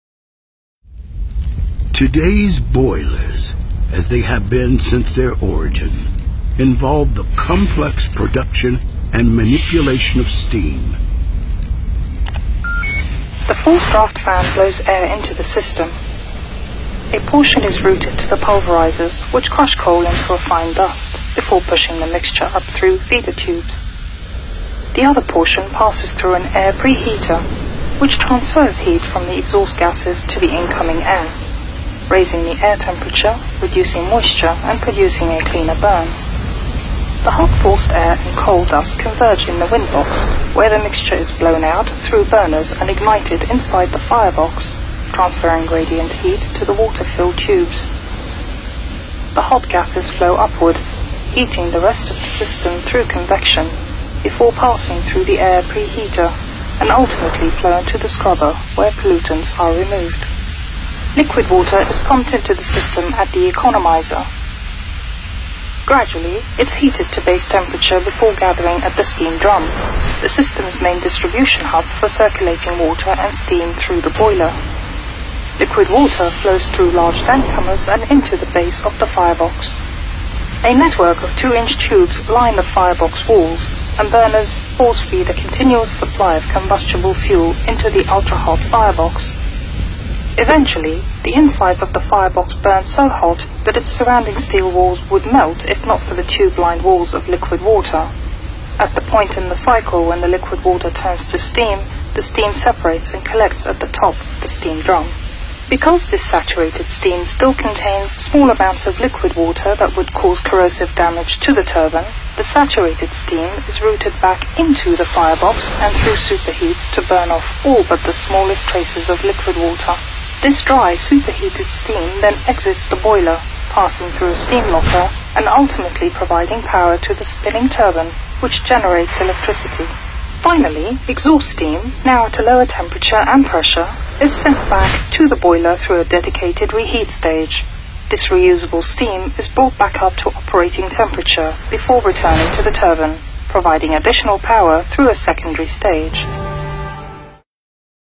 Coal Fired Boiler
This is a simulation of a Coal Fired Boiler for the generation of electricity thru a steam powered turbine. This is not my video I claim no rights to the content. I simply ripped and shared on my channel for education purposes only.